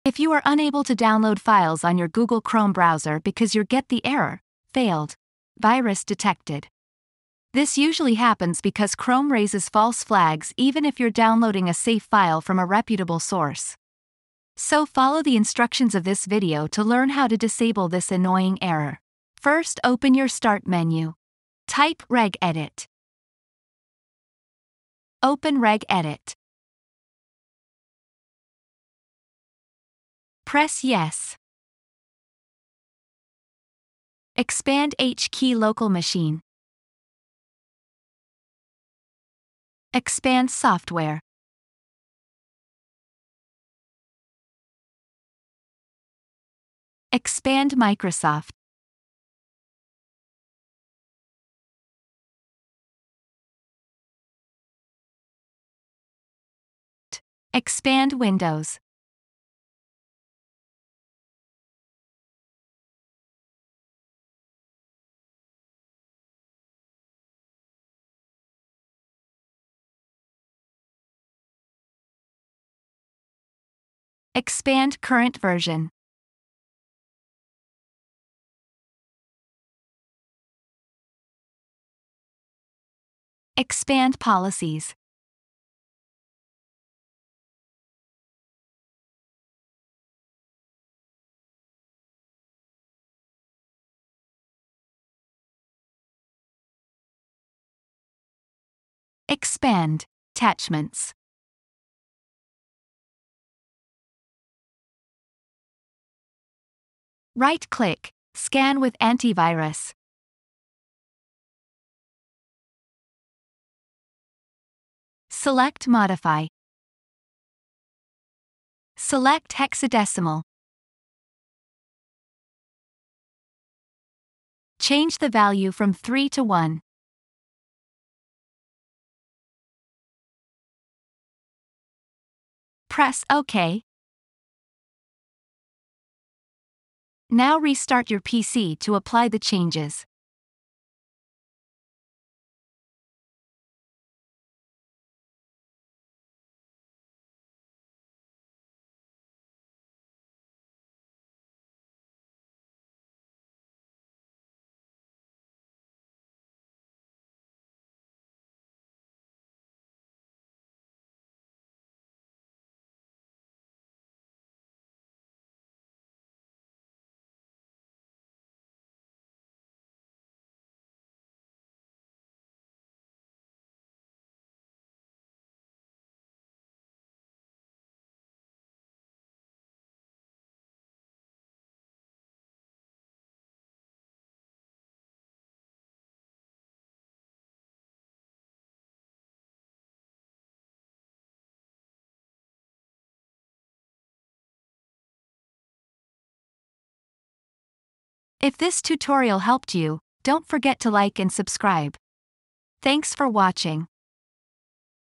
Fix Google Chrome - Failed - Virus Detected - Error Windows - Chrome Download Failed - 2024
If you are unable to download files on your Google Chrome browser because your get the error, Failed - Virus Detected. this usually happens because Chrome raises false flags even if you're downloading a safe file from a reputable source. so follow the instructions of this video to learn how to disable this annoying error.This tutorial applies to Windows 11 and Windows 10.